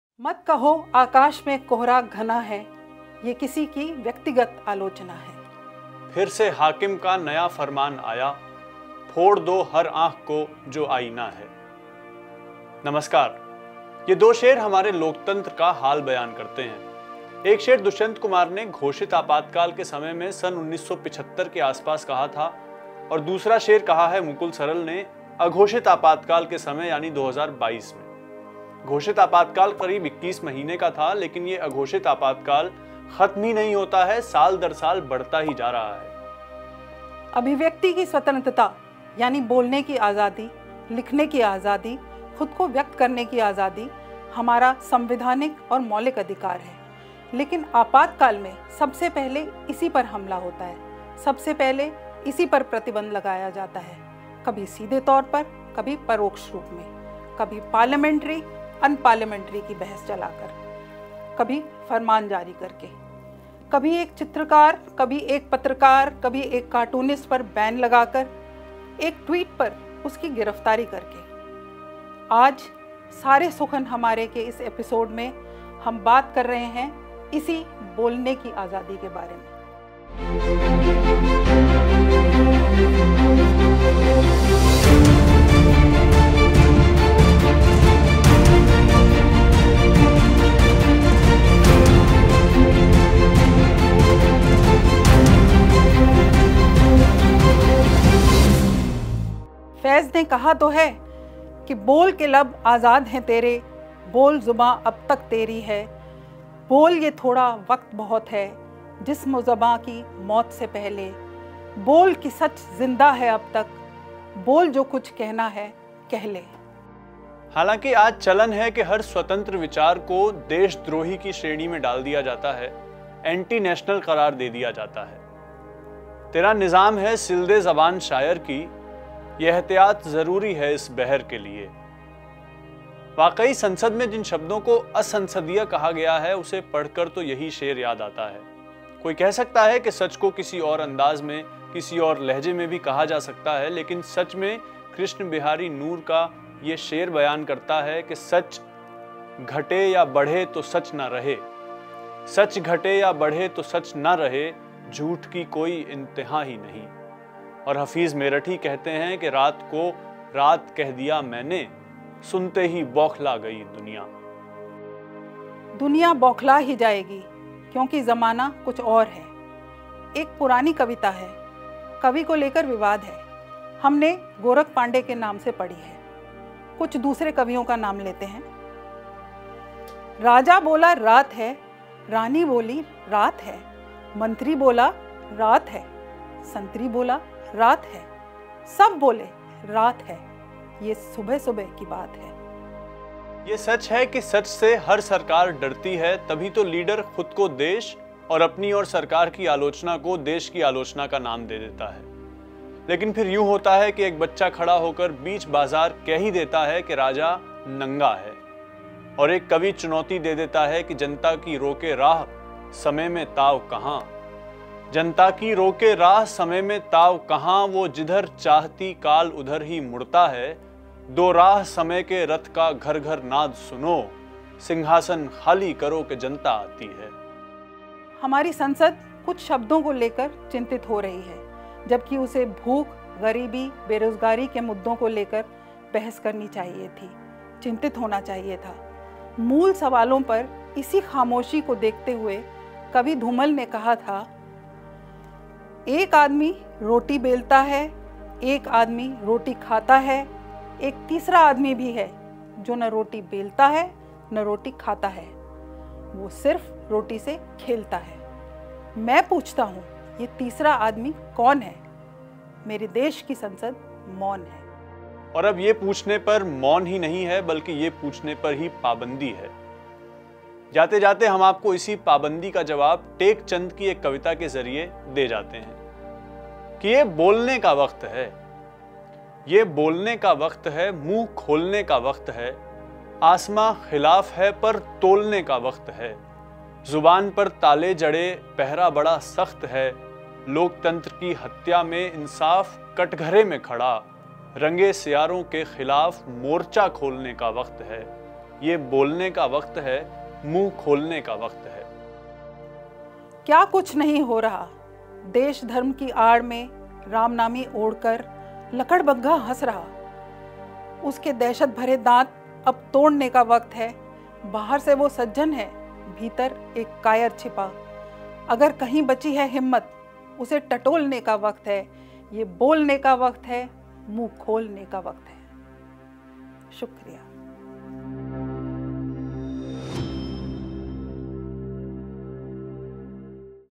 मत कहो, अंधेरा घना है... संसद में सच कहना मना है
अभिव्यक्ति की स्वतंत्रता, यानी बोलने की आज़ादी, लिखने की आज़ादी, ख़ुद को व्यक्त करने की आज़ादी हमारा संवैधानिक और मौलिक अधिकार है, लेकिन 'अघोषित आपातकाल' में सबसे पहले इसी पर हमला होता है।
आज ‘सारे सुख़न हमारे’ के इस एपिसोड में हम बात कर रहे हैं इसी बोलने की आज़ादी के बारे में।